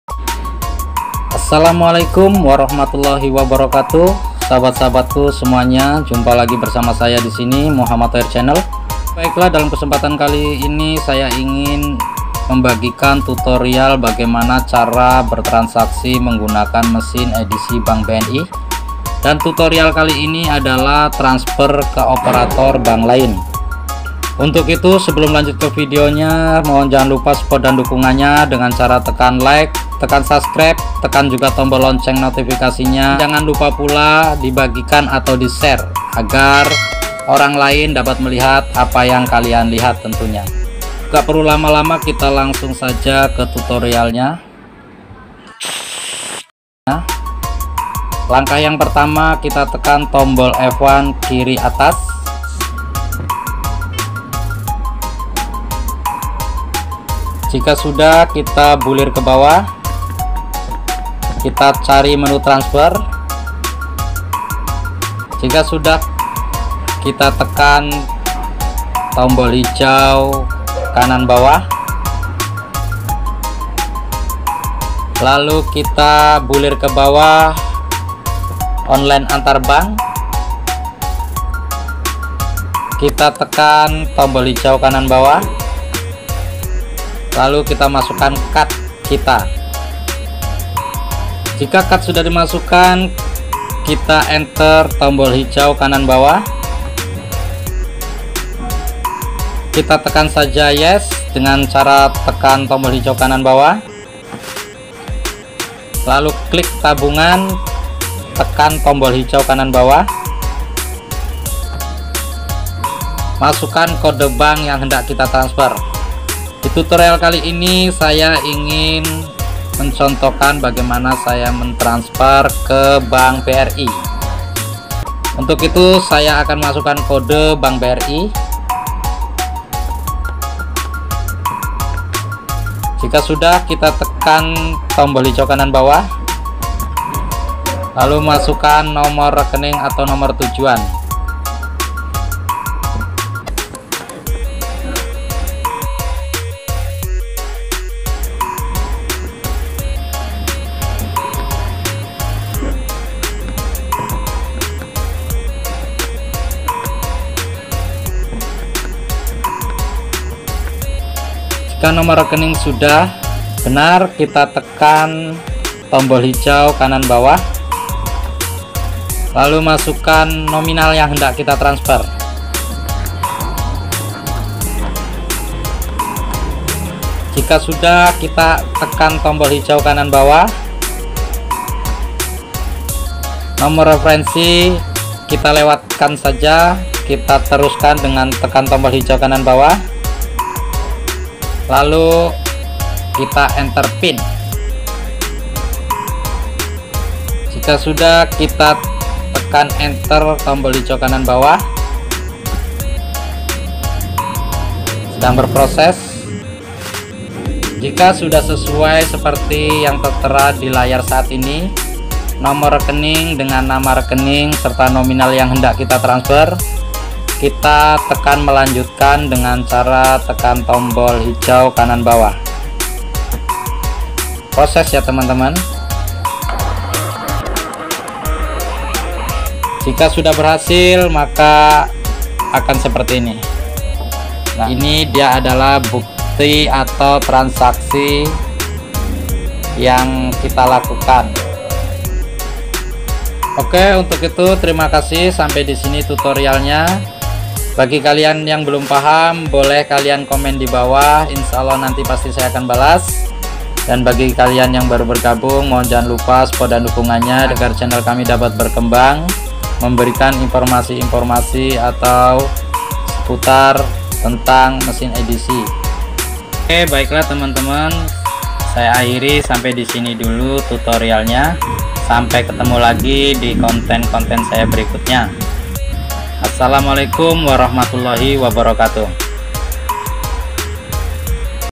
Transfer antar bank dengan mudah
Assalamualaikum semua sobatku 🙏 well come back to my channel, dalam video kali ini saya coba meng info kan
bagaimana cara menggunakan mesin EDC untuk bertransaksi,
dan tutorial kali ini transaksi trasnfer online antar Bank atau yg biasa di sebut transfer antar Bank,
agar tidak gagal faham mohon di simak sampai habis trimakasih 🙏
tinggal kan jejak kalian dengan komen pasti nanti saya balas 🙏

FB HIROSAKA

IG Adeluistoh